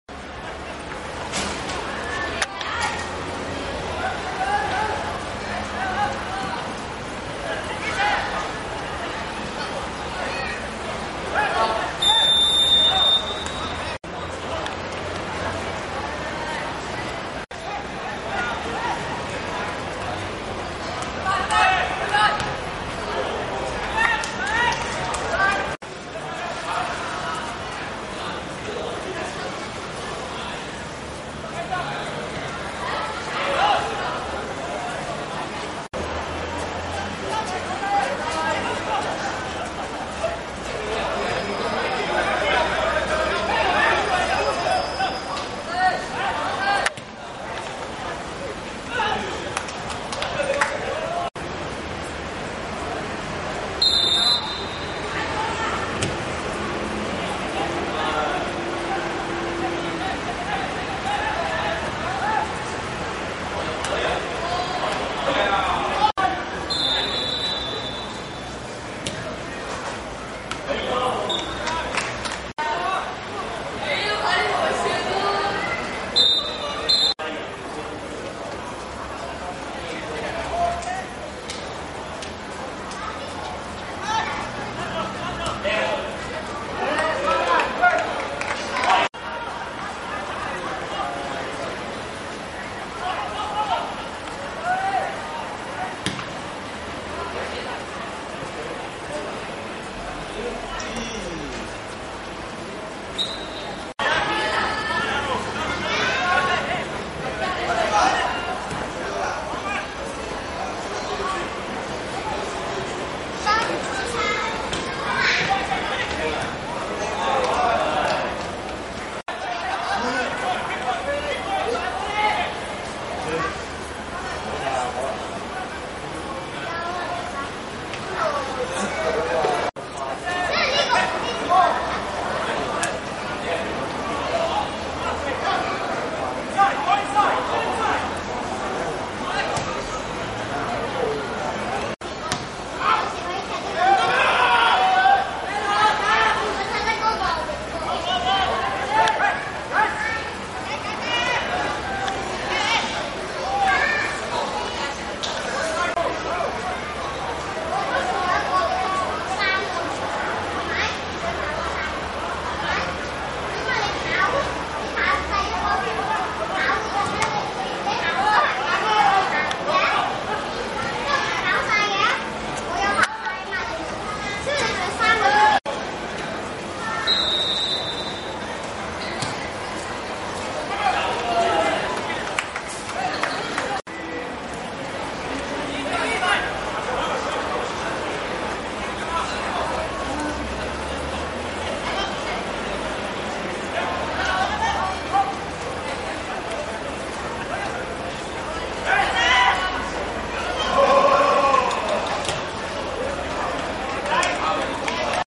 元朗vs流浪(2011季前友賽)精華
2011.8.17.天水圍運動塲 19:30 上半塲流浪入一球 下半塲流浪入两球 全塲賽果 0:3